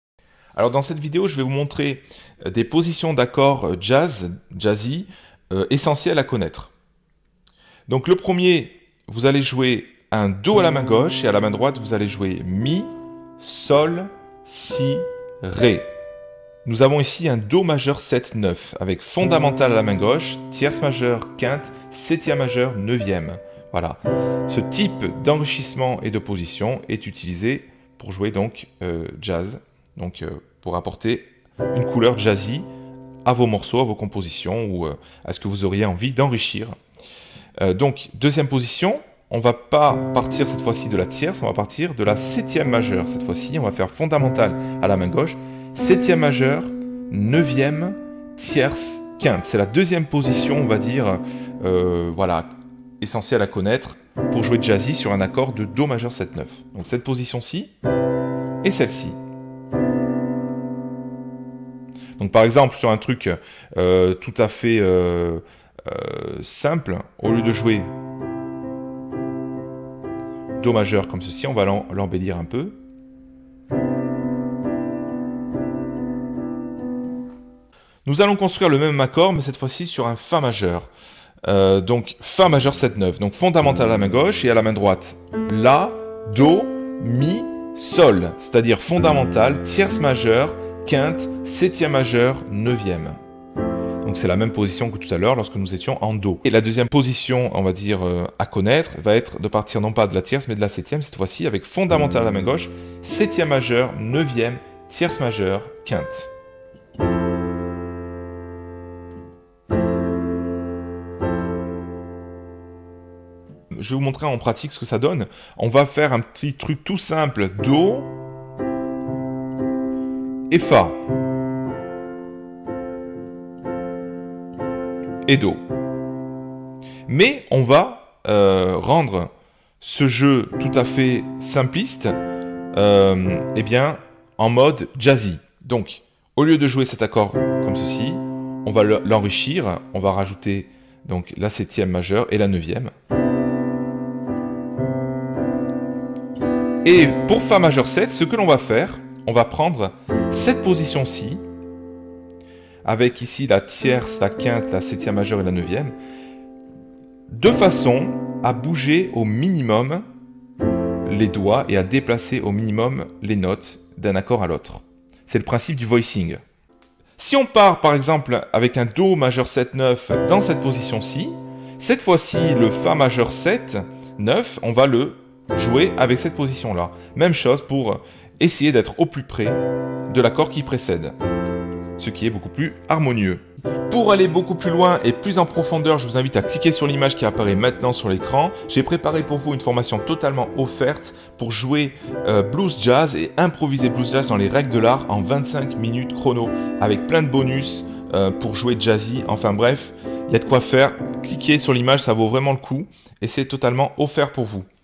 Comment enrichir ses accords au piano et jouer des accords jazz
Dans cette vidéo nous allons voir ensemble Comment enrichir ses accords au piano et jouer des accords jazz
Accompagner au piano avec des accords de 3 sons (triades), peut devenir très lassant...
Il est essentiel lorsque l'on veut embellir son jeu au piano et arrêter de tourner en rond, d'apprendre à enrichir ses accords piano.
Le jazz est un style extraordinaire pour comprendre l'harmonie et libérer ses possibilités créatives.
Avec peu d'éléments, vous saurez comment enrichir vos accords, et épater votre entourage.
Votre jeu ne sera plus pauvre, mais vous saurez comment apporter de la couleur à vos accompagnements.